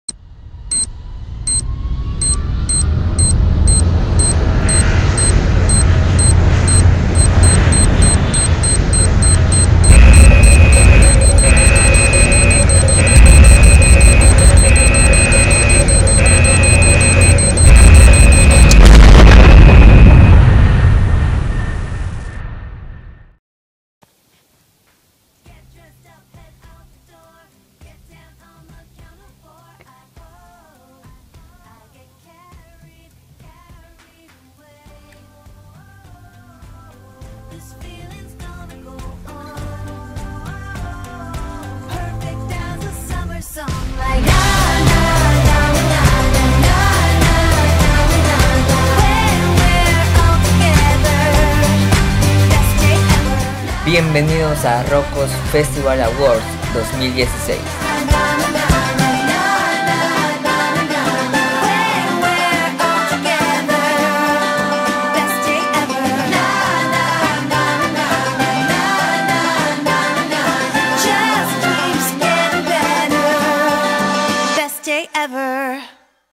Rokos Festival Awards - ADELANTO
Inauguramos el video de un adelanto exclusivo de que se trata los Rokos Festival Awards 2016. Nuestro presentante Rokos ara los honores para presentar el gran Show! Vota, Comparte y Disfruta.

HORARIOS DE LA TRANSMICIÓN:
5:30pm México
8:30pm Argentina
8:30pm Chile
6:30pm Perú
6:30pm Colombia

¡Recuerda que cada ""me gusta" es un nuevo video que se sube!